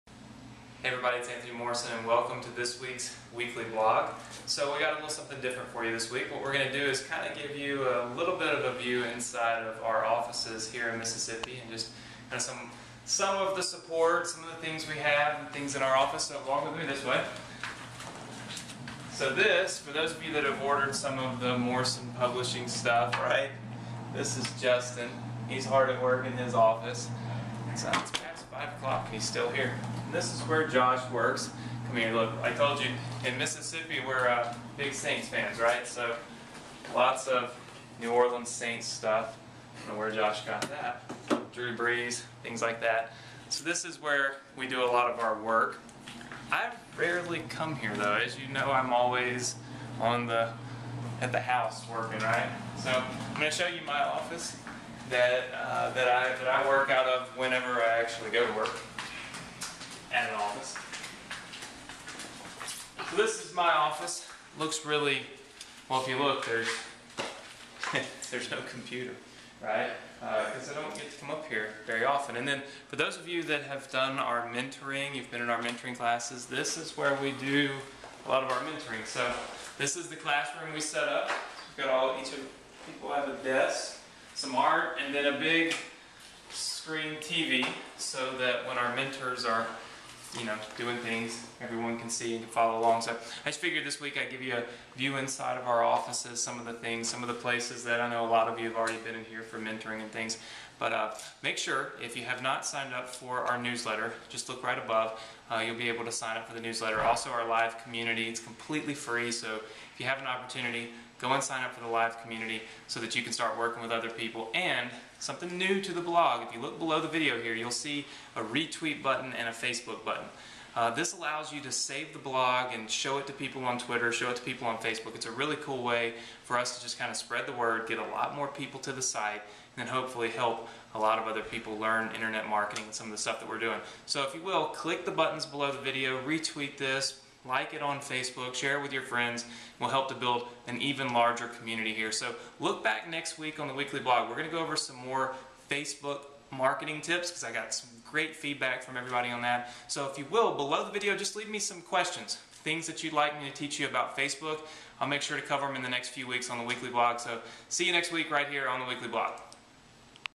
A View Inside Anthony Morrison Office
Just thought I would share a view inside my offices with everyone this week.